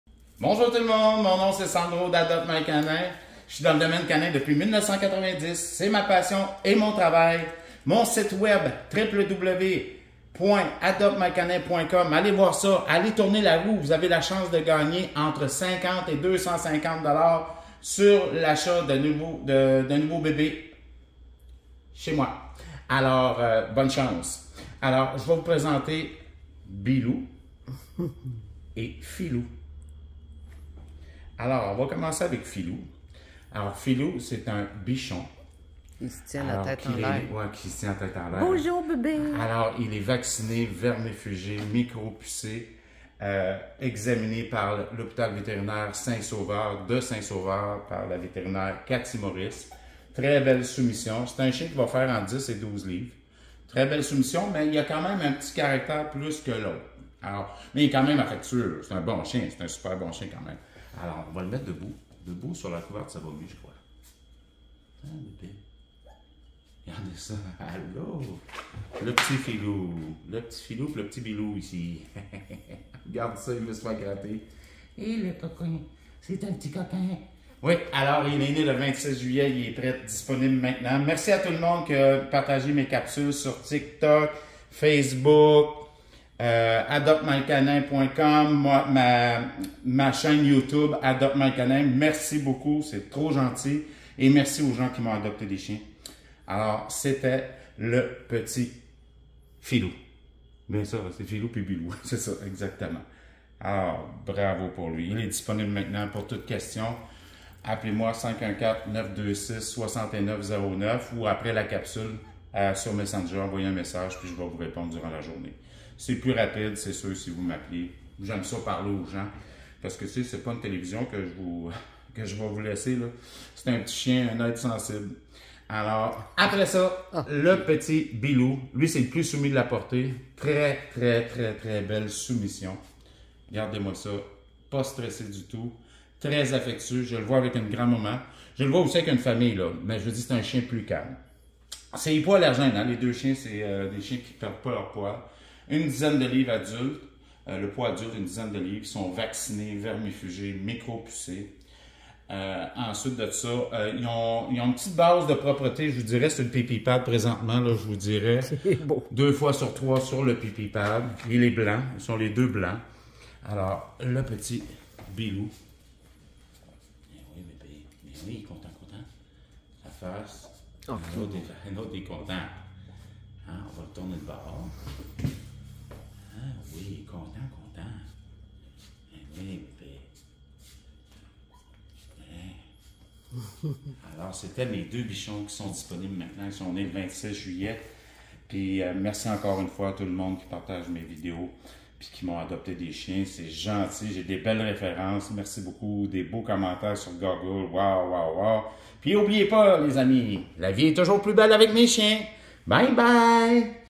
Adoptmycanin présente 2 bichon né 26-06-24 Filou et Bilou disponible maintenant 🥰🥰
Adoptmycanin présente 2 bichon né 26-07-24 vacciner vermifuger micro pucé examen vétérinaire et trousse de départ avec garantie de santé écrite Disponible maintenant contacter nous au Sandro